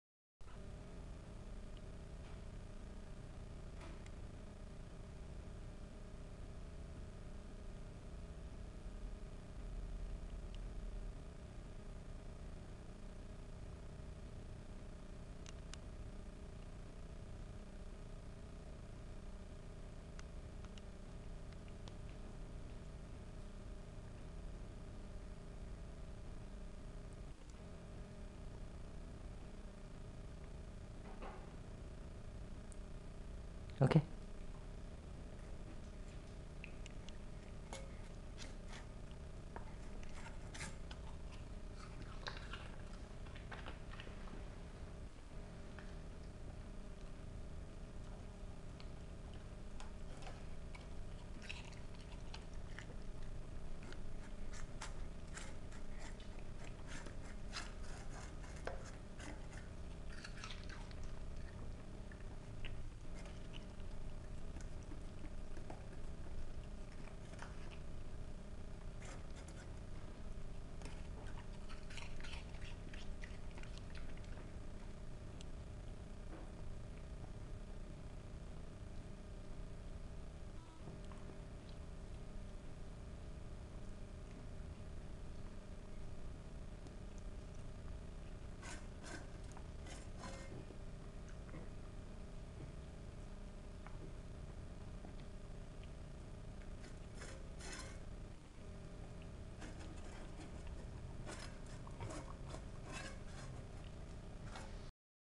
Trinket Eating Apple Slices
Apple slices covered in peanut butter, topped off with green beans. Yum!